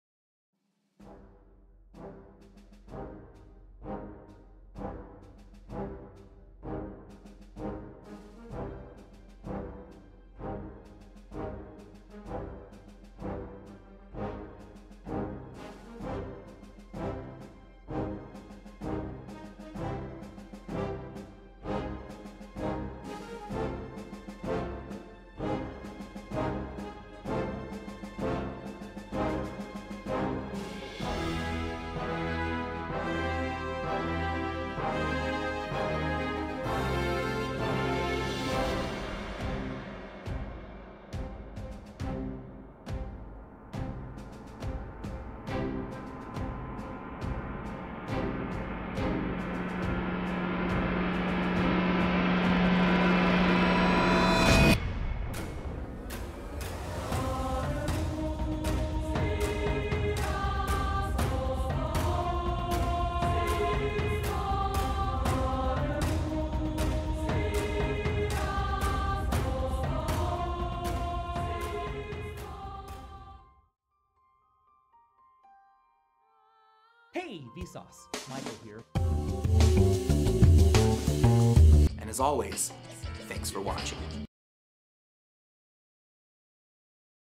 Drawing Michael Stevens from V-sauce . | Timelapse | VSS
Thank you Michael Stevens for sharing fascinating ideas and creating videos that worth watching :)
Sketch Timelapse: (0:00)
Final reveal: (0:56)